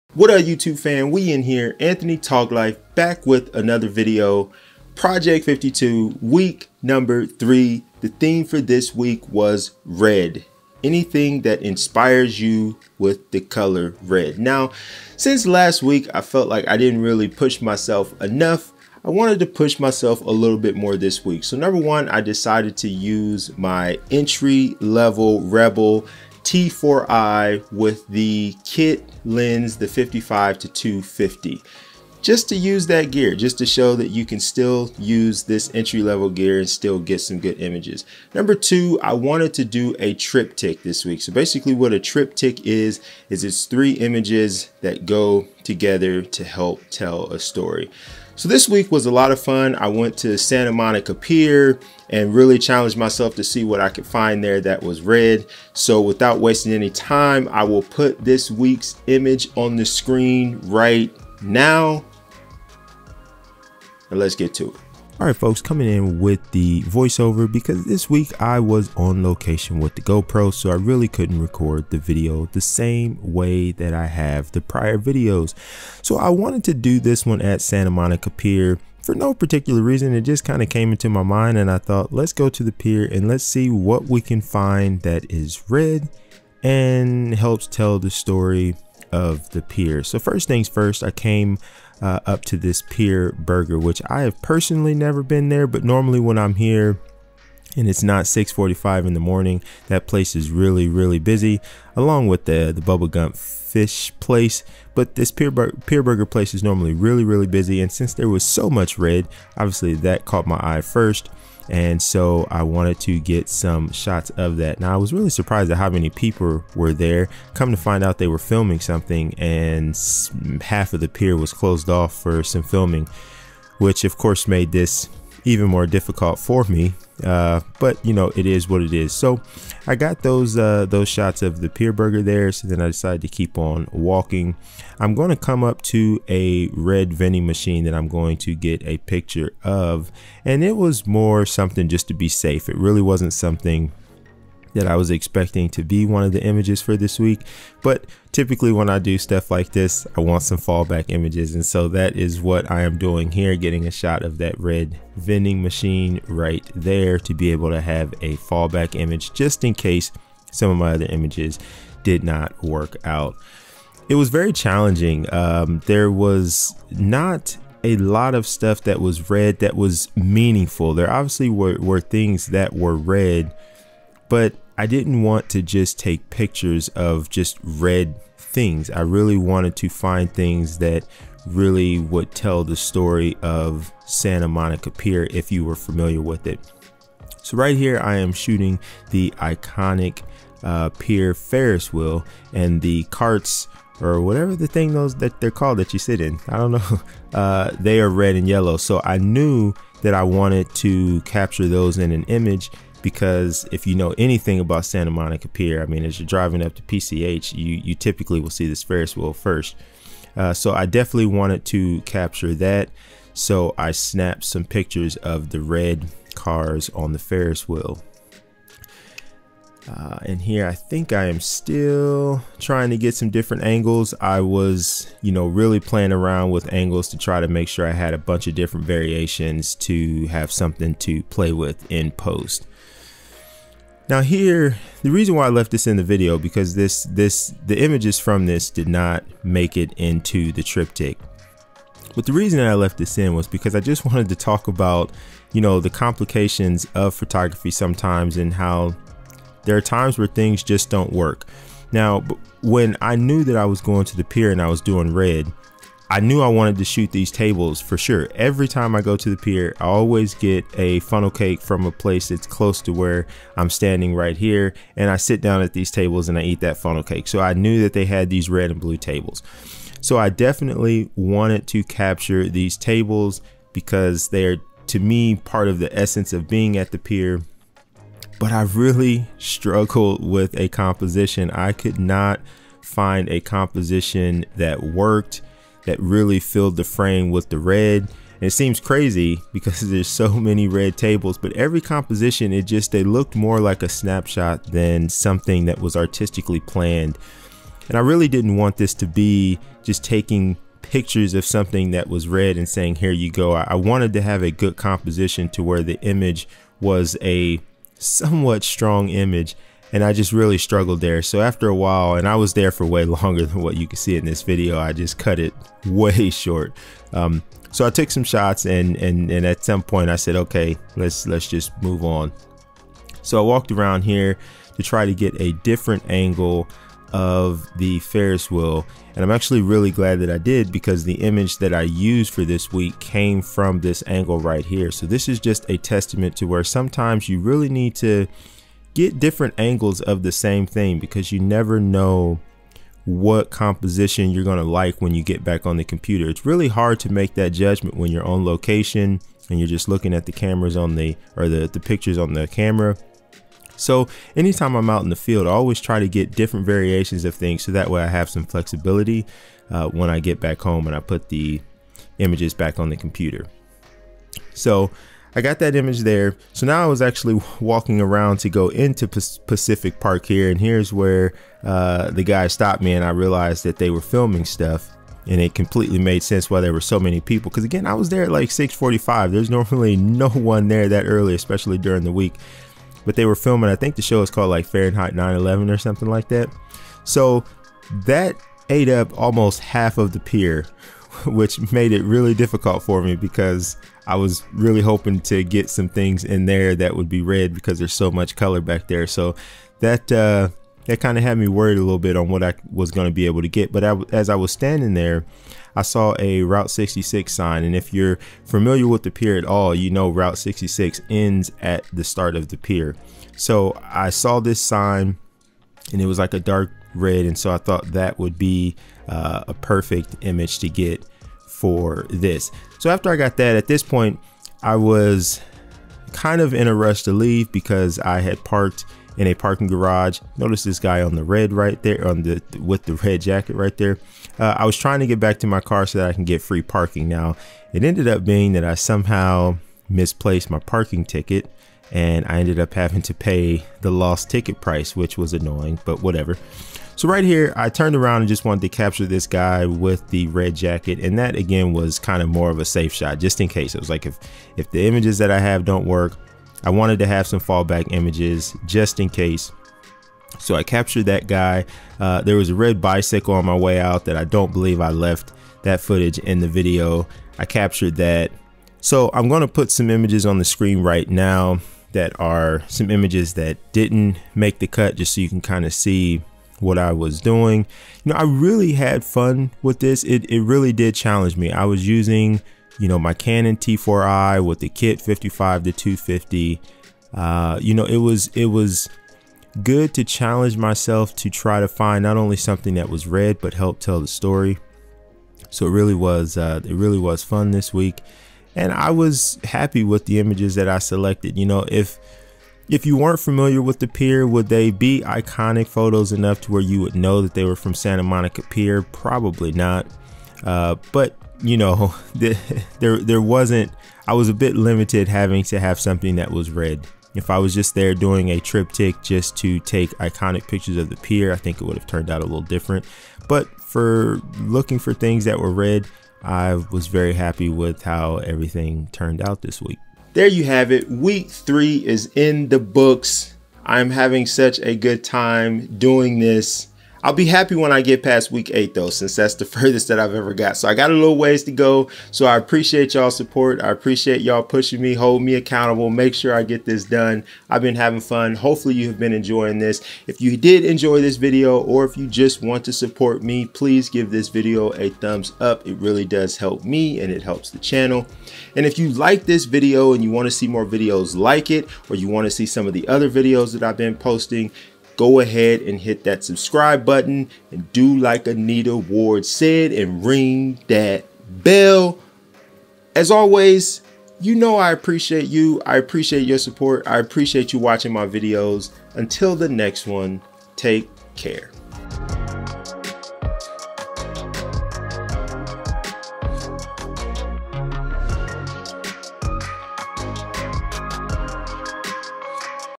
Inspired By The Color Red (Project 52)
Welcome to week 3 of my project 52 weekly photography challenge! This time around I take you along with me as I'm capturing this week's image and provide my commentary/thought process behind the photo.

The theme for this week is:
Inspiration: Red | Shoot whatever inspires you. Red should be the focus of the image. Don't be afraid to be creative.

Canon 24-105mm f/4L USM
Tascam DR-10L Portable Digital Recorder

[f/16 Photography Podcast]
►Anchor: anchor.fm/anthony-toglife